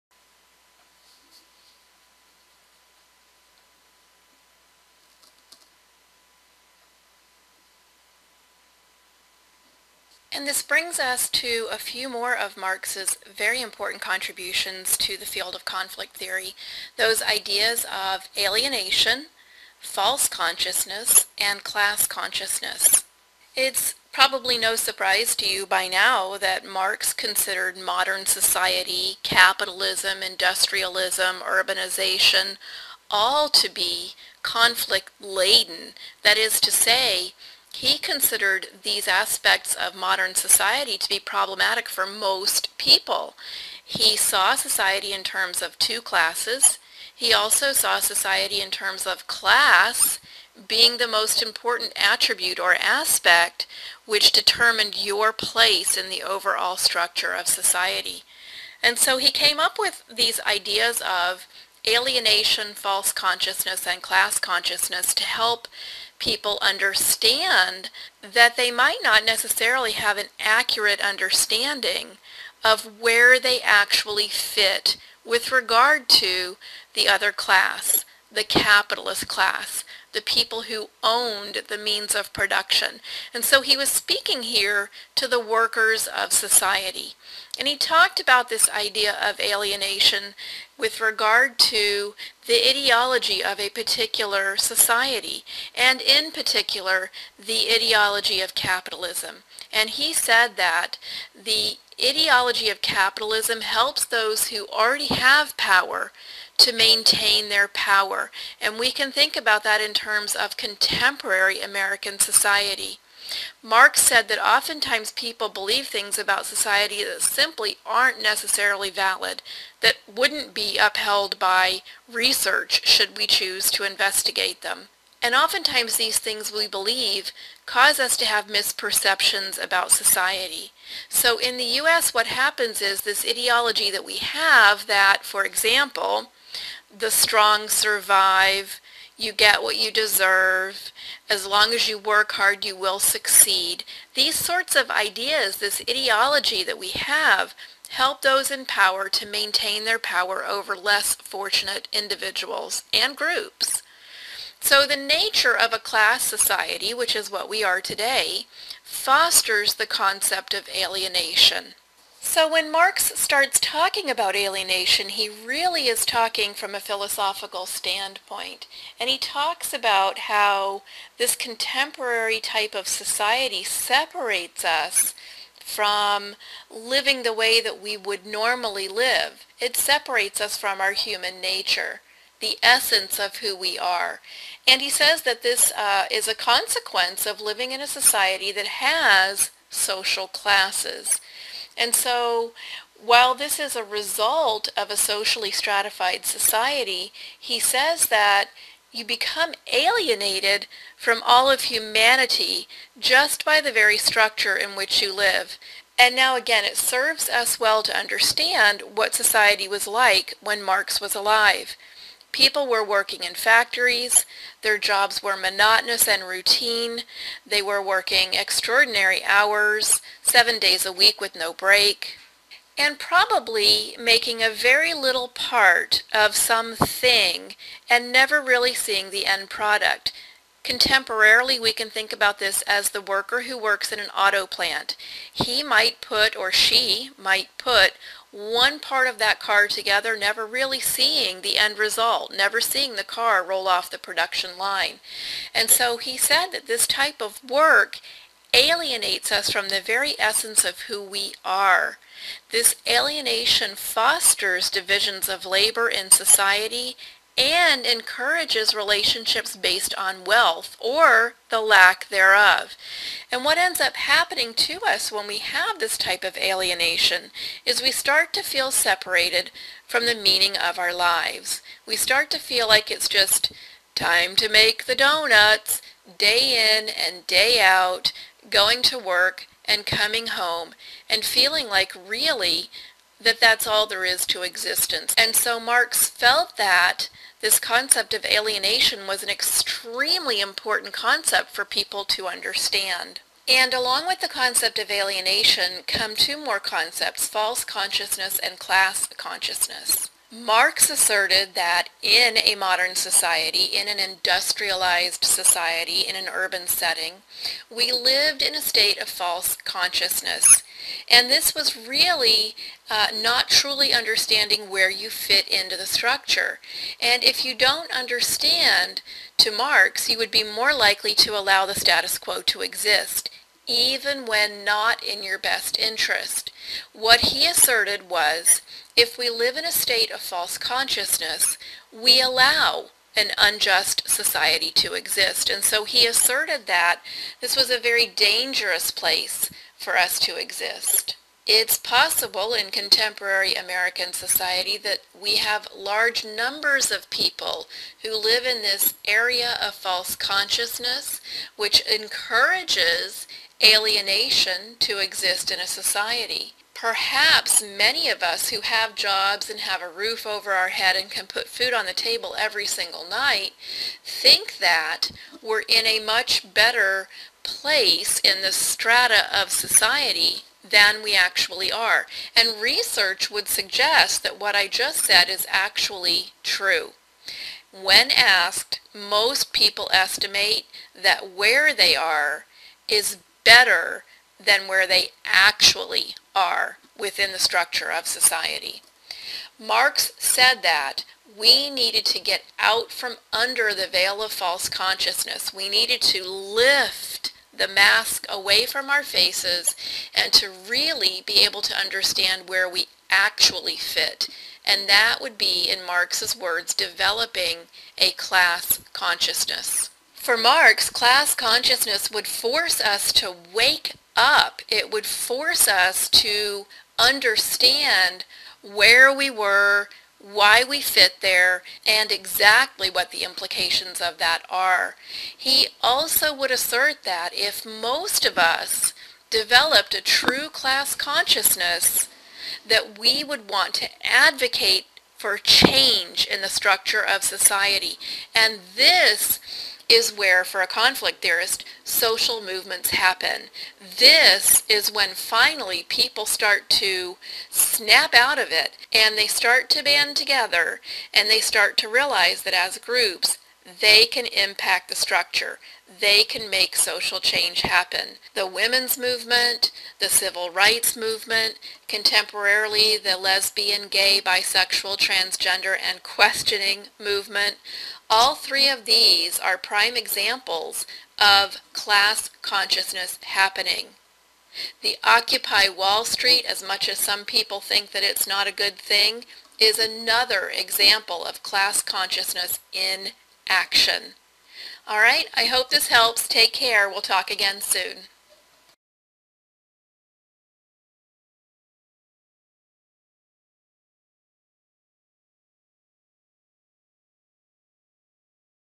Karl Marx - Alienation and False/Class Consciousness
Marx felt that people's labor could be transformed into a power which could rule them like an automaton. Developed in 1844, the concept of alienation furthers Marx's critique of capitalism.

Marx also believed that the working class had the "wool pulled over their eyes," and were living under a veil of false consciousness. He said that only when this veil was lifted would the working class develop a true sense of how they fit into the hierarchy (stratification) of society. When the veil of false consciousness lifted, the working class would unite and work to better their condition, ultimately overthrowing the government and the ruling class to create a more egalitarian society.